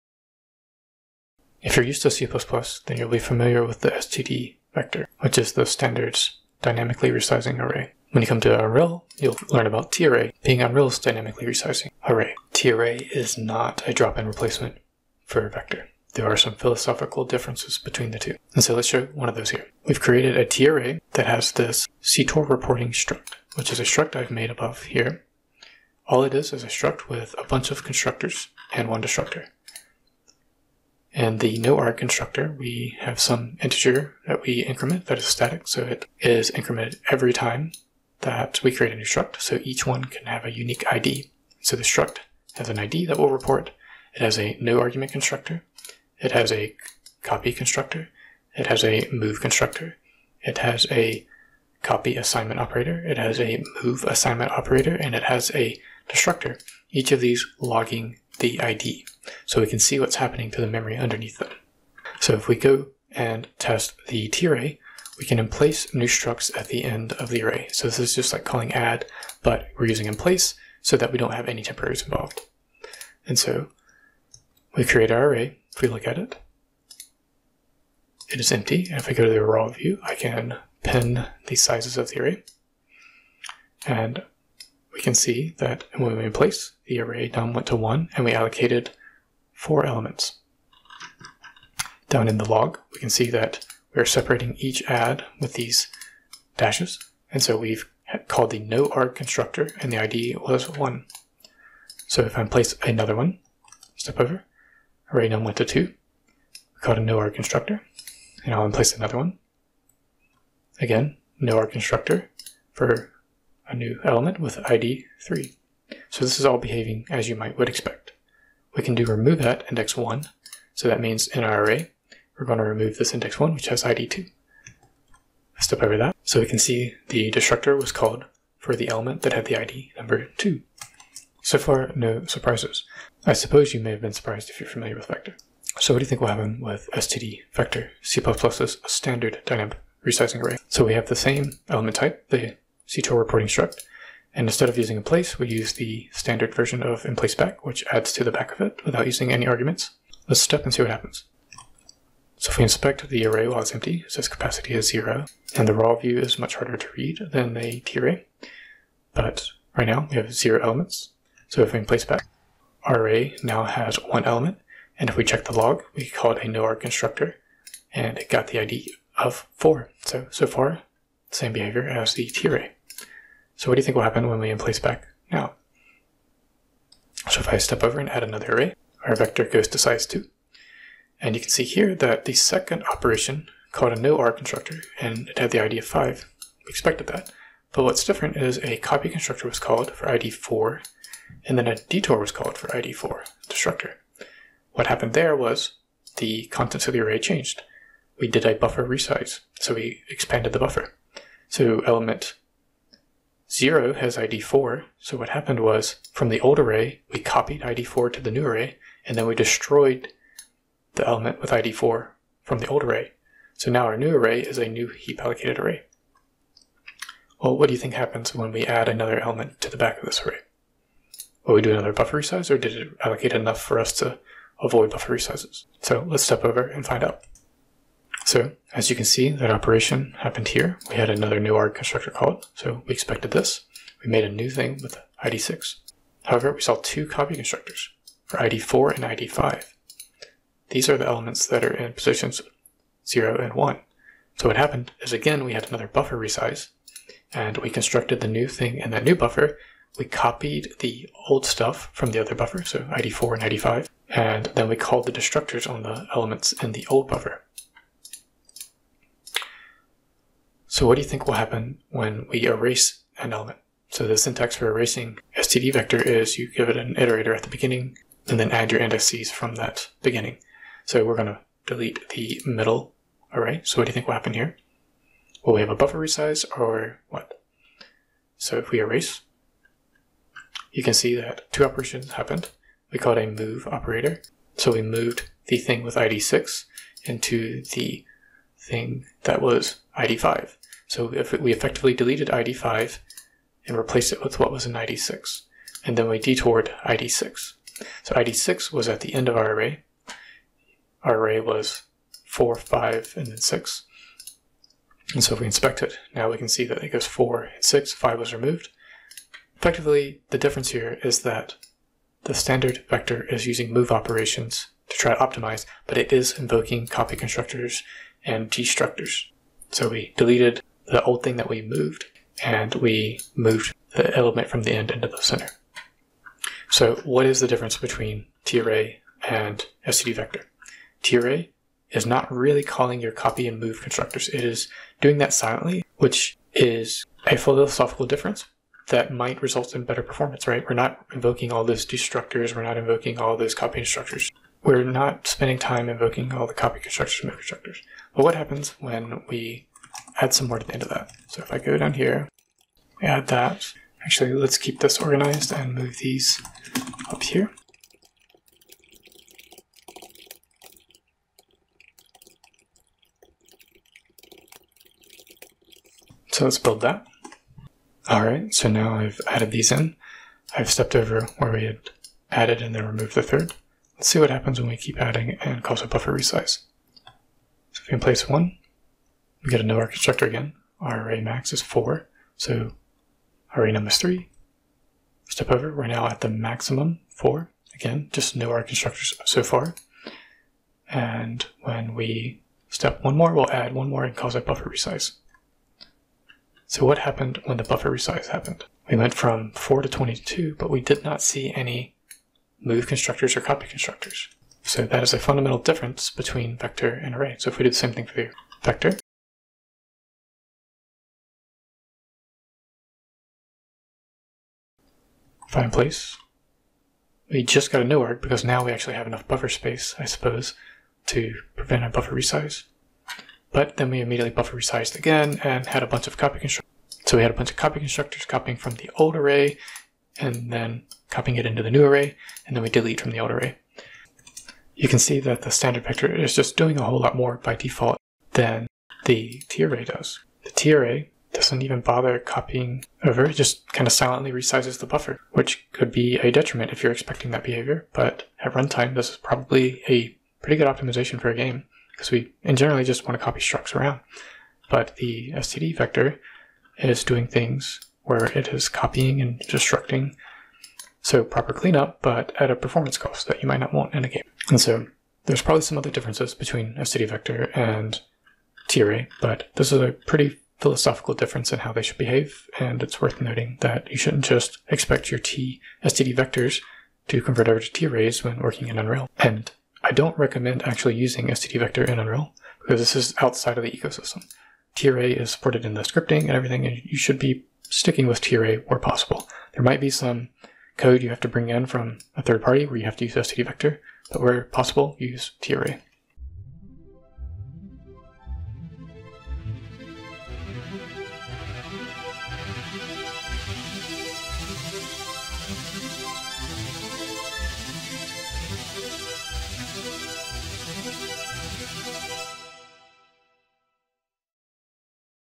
TArray vs std::vector - Unreal C++ - UE C++ Tutorial
std::vector and TArray are similar, but they're also very different.
TArray is built for games, and it does raw memory copies during buffer resizes.
std::vector is built for general purpose c++ programming. It invokes copy, move, and destructors during buffer resizes.
This means that TArray ultimately is using less cycles on a buffer resize, making it potentially faster.
std::vector also does more buffer resizes for the same number of elements added.

However, this means that certain patterns in c++ might break with TArray.
For example, if you use references or pointers internally to your class, and you use it by value in a tarray, you may have dangling pointers.
As the TArray will copy and paste the memory into the new buffer, which will have a different memory address.
However, in practice within unreal this is not common. Because most pointers leave on the heap, so it is fine to do shallow copies.
But it is important to be aware of this behavior, so you don't accedentially write dangerous code.
I recommend using TArray over std::vector while in the engine.
It is fully supported by the scripting system, and heavily embedded as part of the Unreal ecosystem.

0:00 TArray and std::vector are similar, but difference
0:21 Custom struct that reports constructor operations
1:01 What happens when we dynamically add and remove elements with TArray
2:15 What happens when we dynamically add and remove elements with std::vector
5:10 Comparing std::vector and TArray in terms of constructors called
5:45 Testing what happens during a TArray buffer resize by adding a 5th element.
7:15 Summary
8:15 Outro